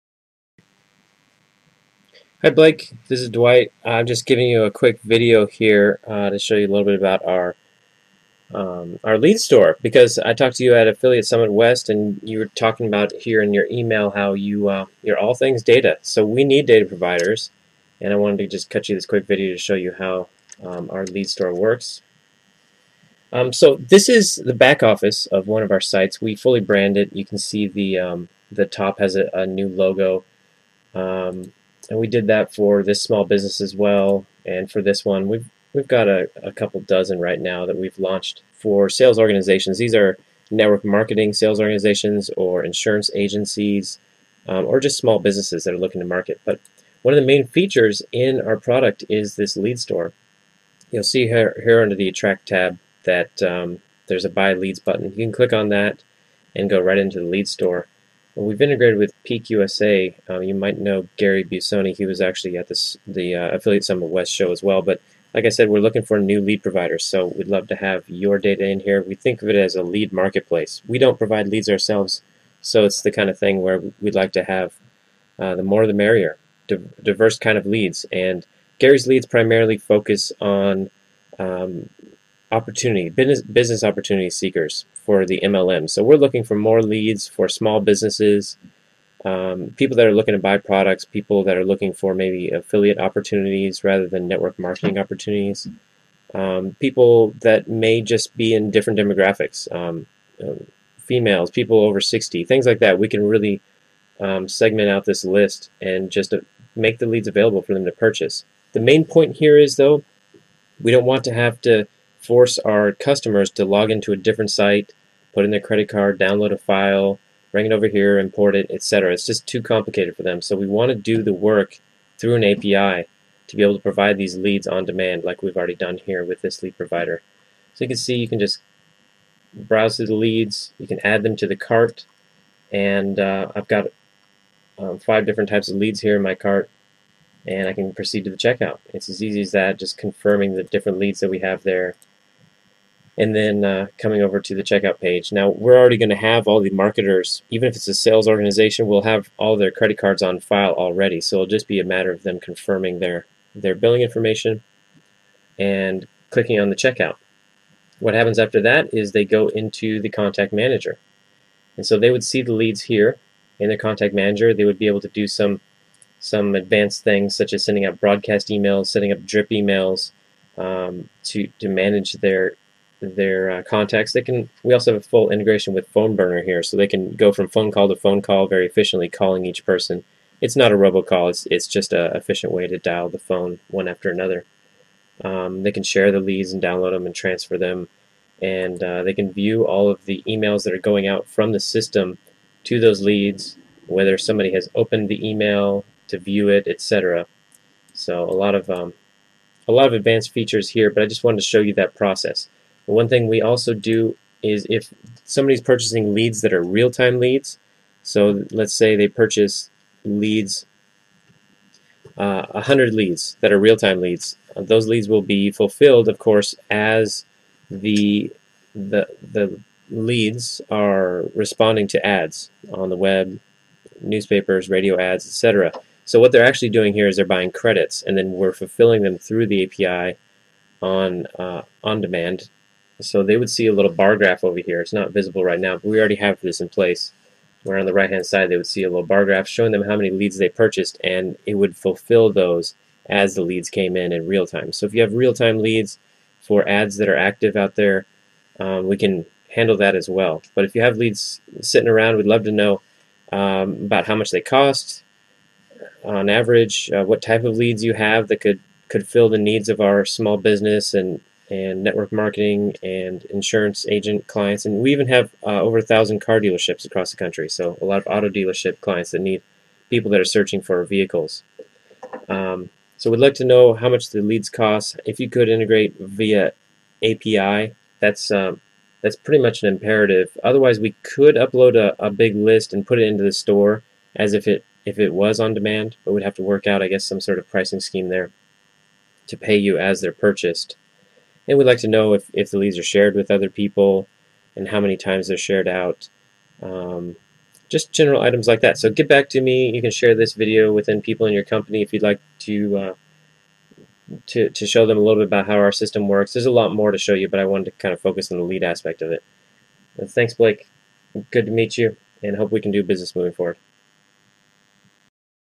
ApogeeINVENT Lead Store Review - Right1Data
Video for Blake with Right1Data. Quick review of ApogeeINVENT Lead Store and Contact Manager.